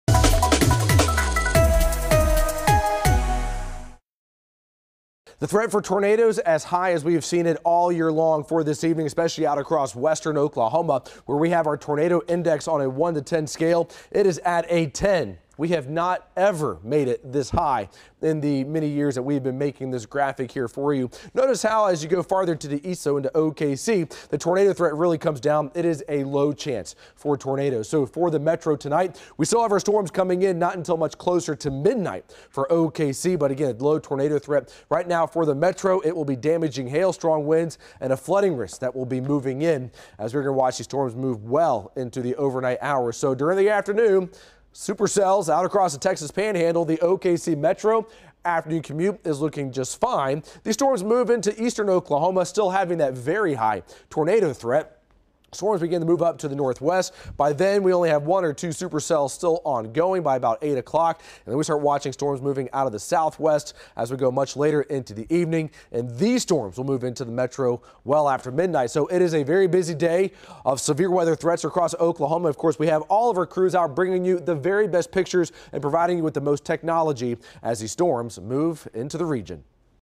Tornado Risk VERY HIGH for Tuesday night
Chief Meteorologist Damon Lane shows the tornado risk for tonight